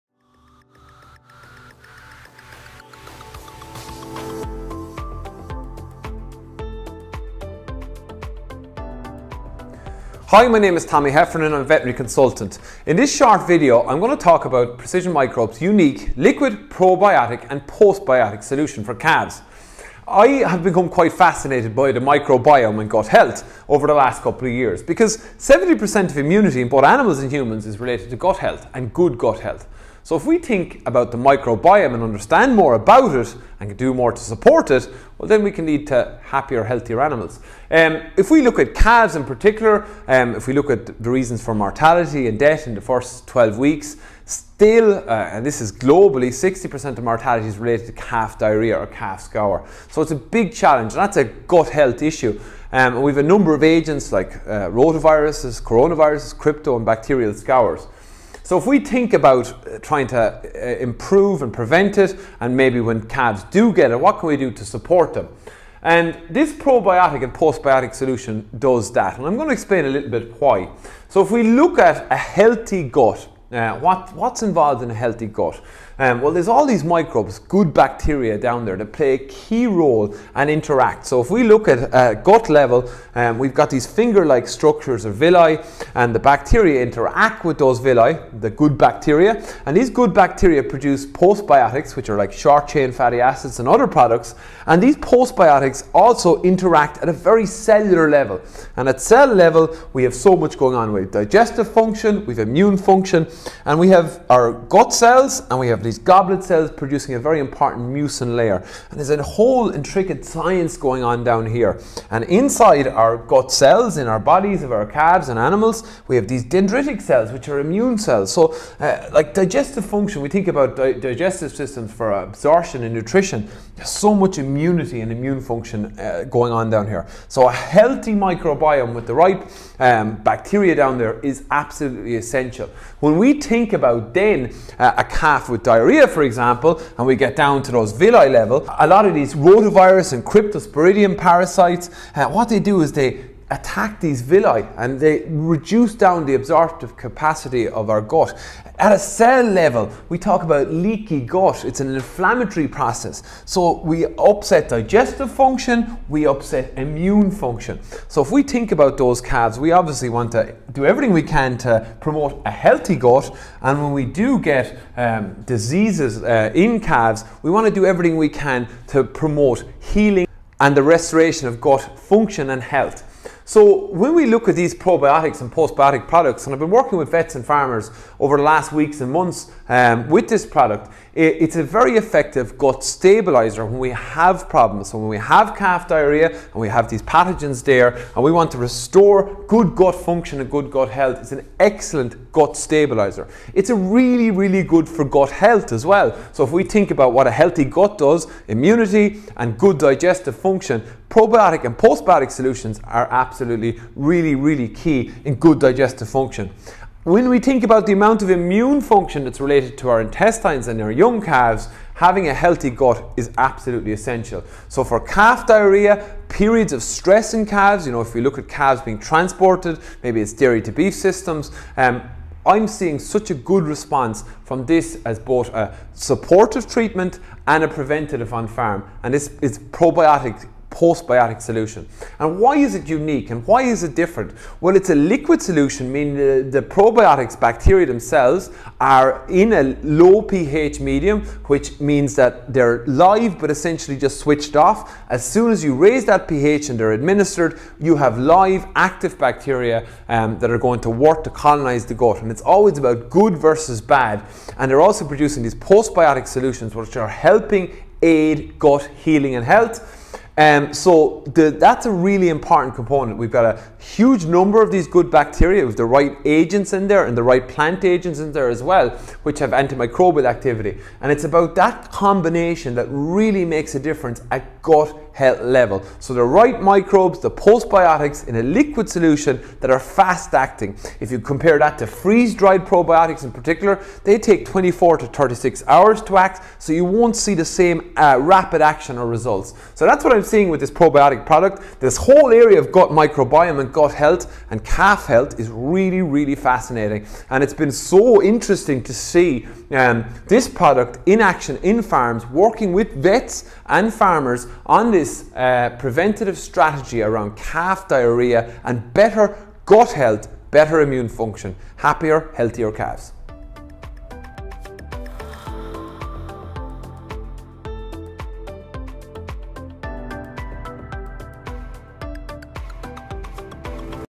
Precision Microbes Calf Probiotic & Postbiotic
Precision Microbe’s Calf Probiotic & Postbiotic
Seventy percent of the immune function is thought to be in the digestive tract of animals. We at Precision Microbes know how important a healthy gut is in young calves.

The immunity in the gut is controlled by the billions of microbes (the microbiome) that interact with local intestinal cells and the immune system.

A healthy microbiome ensures that digestive function and immune function are working optimally. Our team of experts has created a unique probiotic and postbiotic liquid solution to ensure that calves achieve optimal gut function and health.

Our formulation of essential plant extracts, good bacteria, and postbiotics provides an excellent gut stabilizer for calves’ recovery from digestive upsets.

Fast action
This liquid probiotic solution provides a high concentration of live bacteria that immediately stabilize the calves’ gut microbiome.

These live bacteria mean rapid action compared to freeze-dried products, which can take up to 24-36 hours before they are active.

The feedback from vets and farmers has been very positive since we launched into the Irish market.

A veterinary surgeon must assess each case or outbreak.

We see issues with viral, bacterial, and cryptosporidium parvum. Our product can help aid and speed up recovery.

Why it works so well?
The solution does this by containing both the right blend of probiotics and postbiotics. The probiotics help heal the gut lining and bind pathogens. It also helps quickly restore good local immune function.

In gut stabilisation, our unique blend of plant extracts and postbiotics, like short-chain fatty acids, is beneficial to gut health.

Our expert team is taking years of experience in microbes, working with vets and farmers to find innovative natural solutions to some of our biggest on-farm challenges.

Calf diarrhoea accounts for up to 60% of all mortality in young calves under 12 weeks. We can play our part to improve the welfare and the health of calves by focusing on good gut health.

Any questions, please contact our team at Precision Microbes.